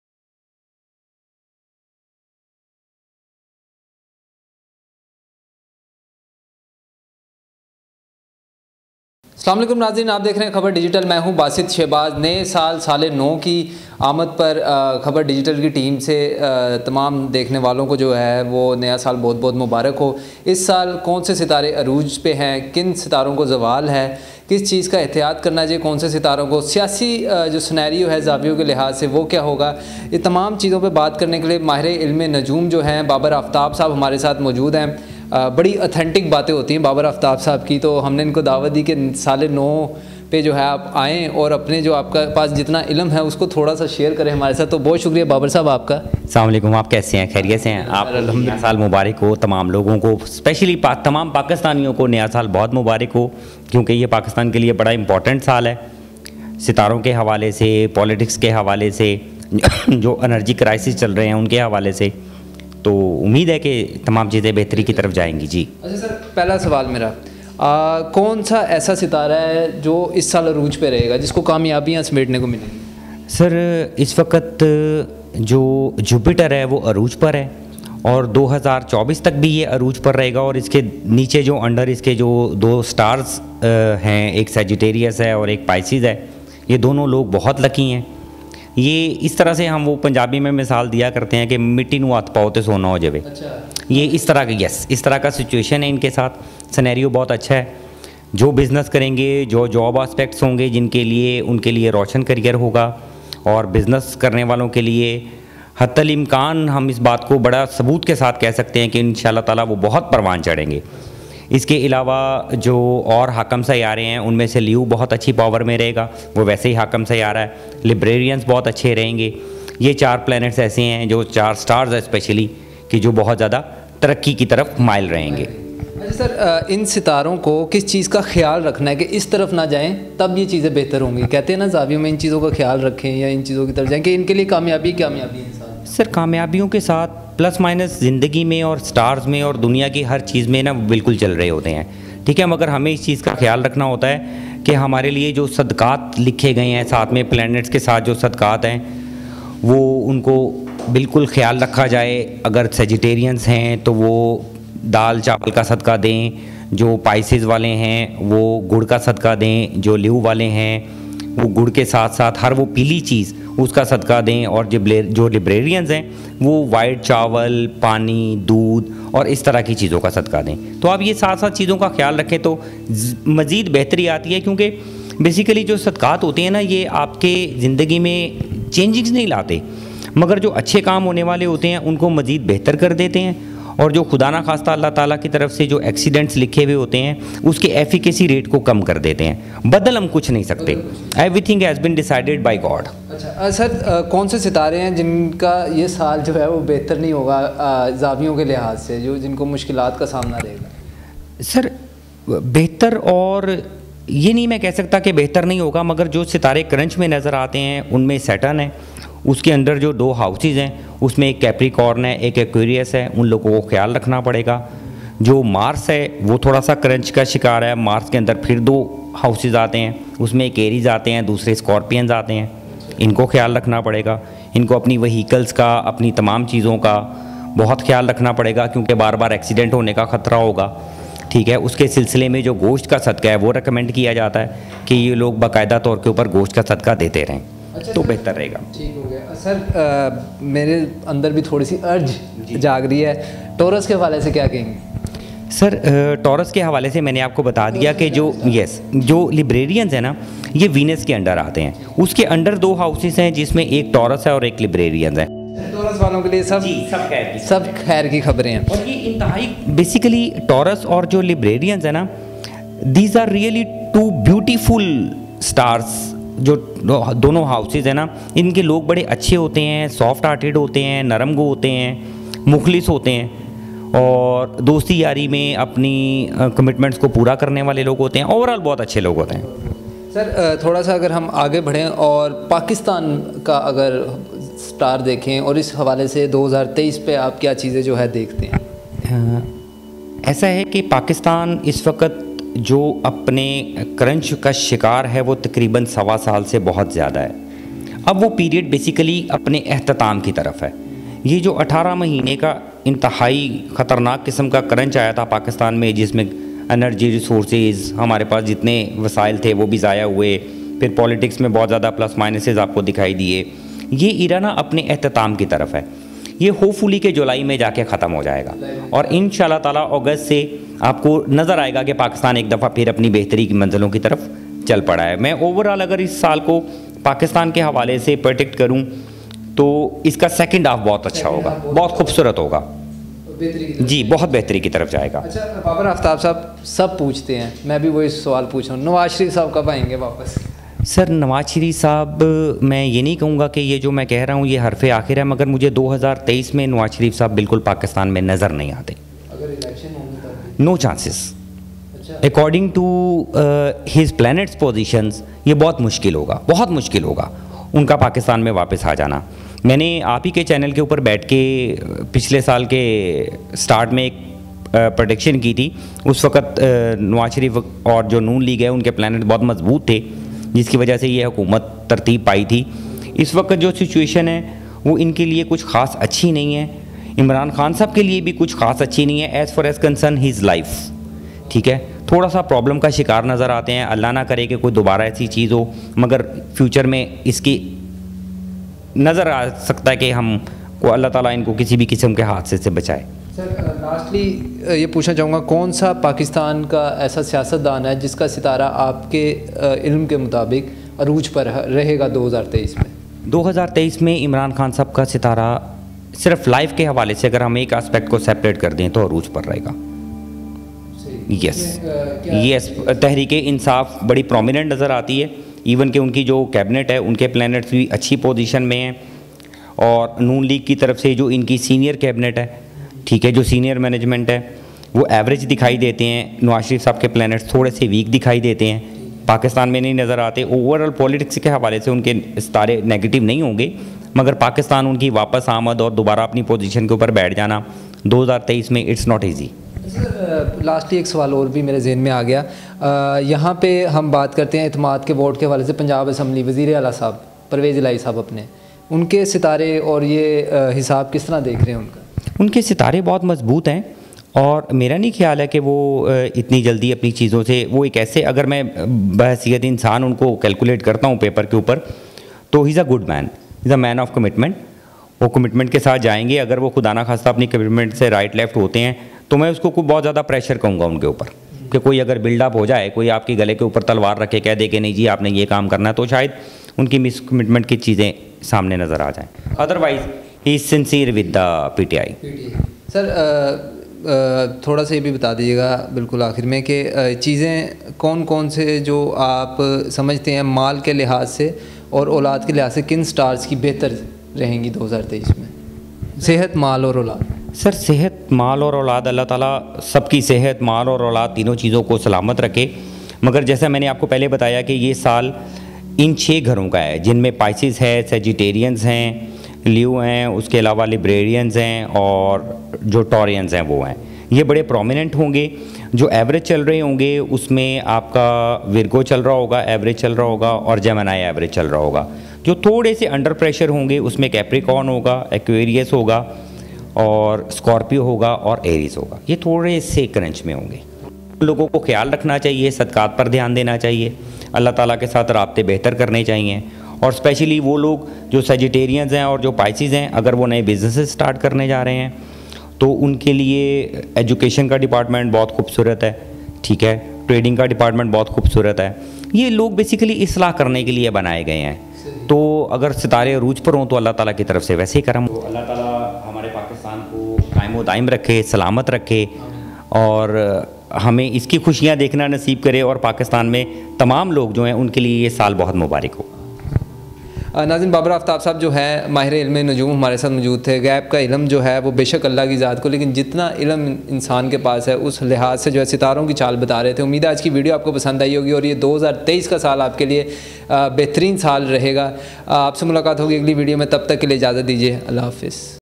Prediction of 2023 By renowned astrologer Babar Aftab| Interview| Khabar Digital
How will be this year 2023, What is the political scenario in 2023 and How's will raise in 2023 told by  Babar Aftab. Khabar Digital host  Basit Shahbaz interviewed  a renowned astrologer Babar Aftab in which he told about the horoscope.

Host: Basit Shahbaz